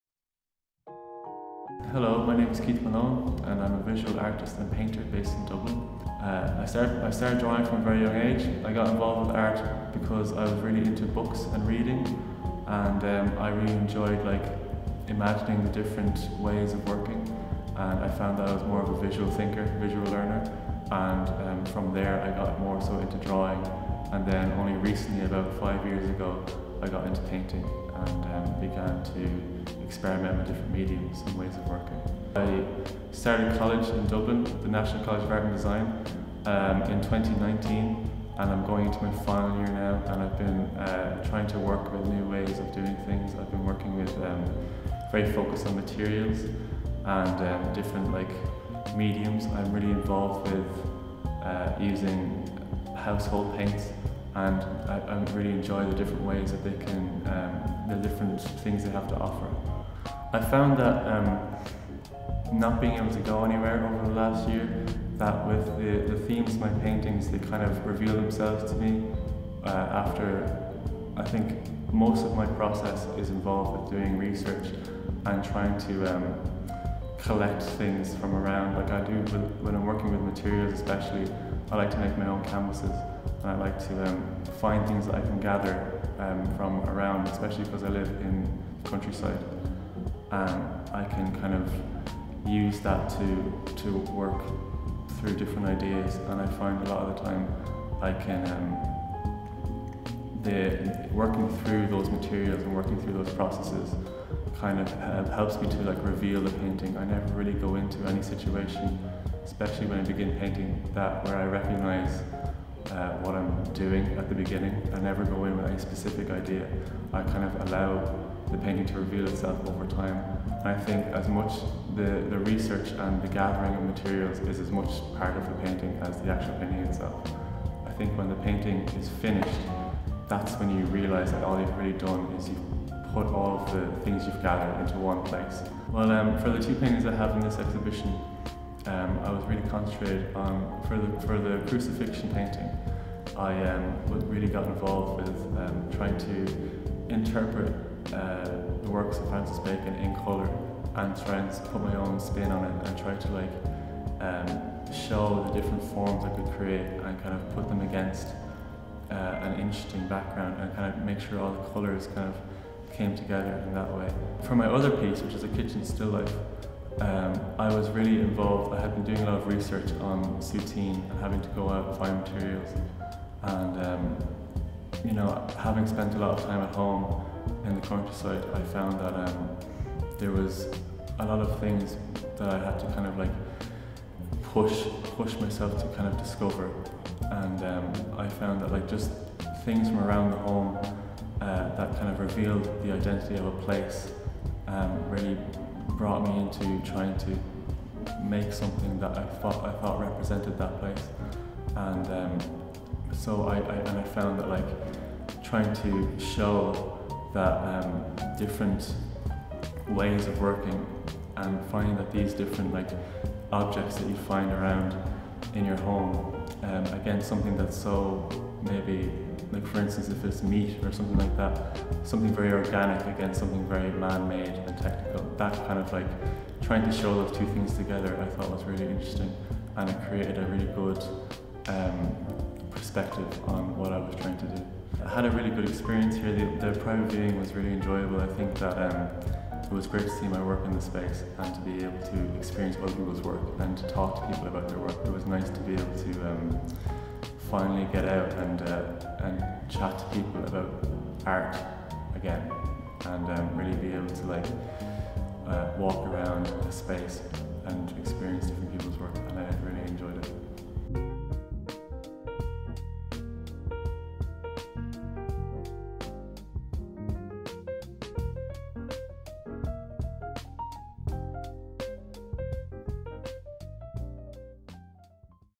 The Holy Art meets Keith Malone
Keith Malone is a multidisciplinary artist based in Dublin, Ireland. Preferring to work on a large scale, he views his works as heavily researched investigations into the structure of art and art history. Focusing particularly on the nuances of artistic identity and consciousness, as well as interrogating the merits of expression and critiquing the institutional values placed on art.

Material plays an important role in his work. He constructs his paintings from scratch, often using found materials and wood, as opposed to canvas, because it allows for rougher handling of materials as well as accommodating harsher, more destructive mediums.